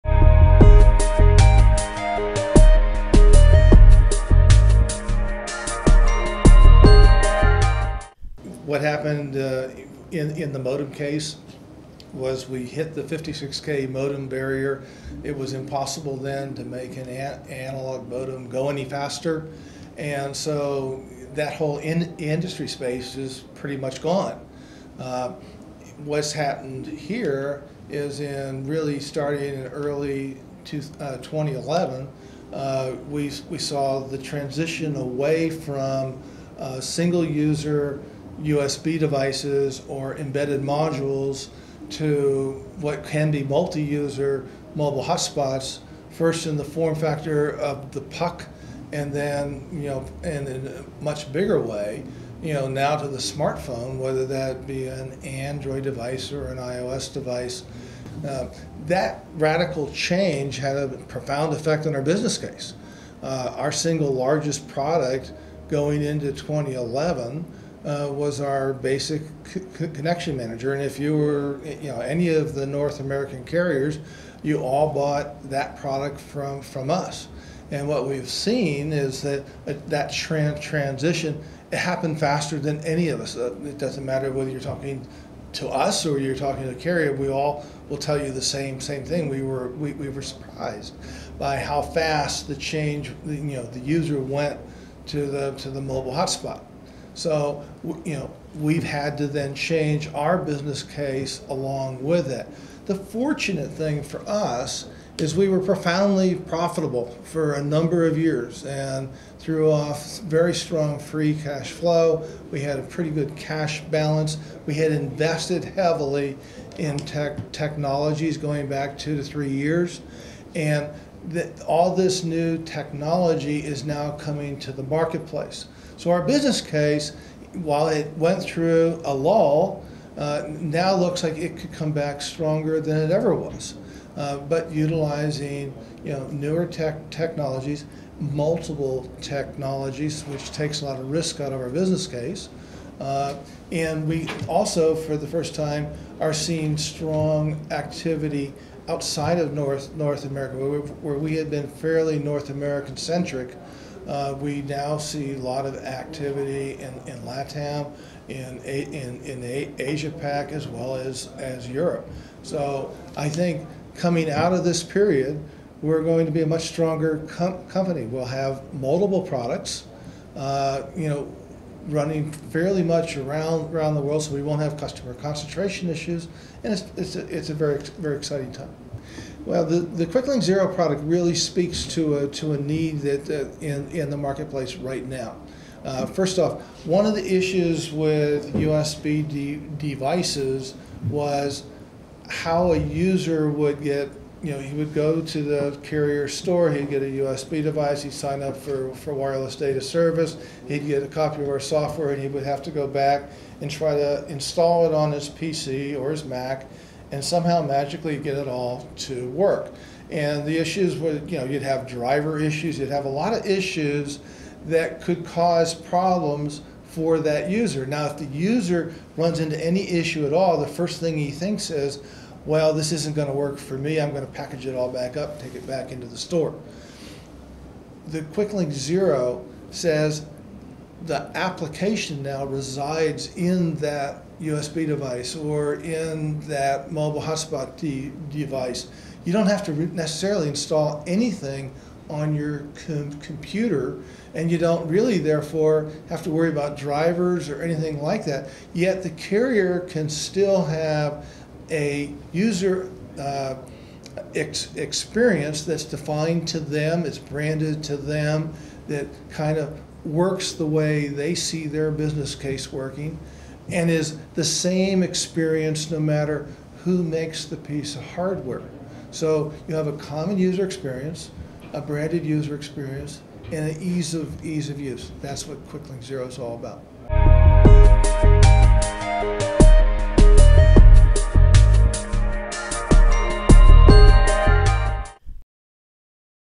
MWC2012: The evolution of connection manager software and impact on Smith Micro bottom line
William Smith, Jr., Smith Micro CEO, provides historical perspective of connection manager tools from 56k analog modems to smart phones and mobile hotspots.

The rapid user adoption of WiFi hotspots caught many by surprise including Smith Micro and mobile carriers.

Smith Micro's existing cash balance resulting from long term operating cash flows allowed them to invest in new products and international growth.

Quicklink Zero product solves problem of loading connection manager software on PC, Mac or smartphone.  The application resides on the hot spot allowing carrier to personalize customer experience.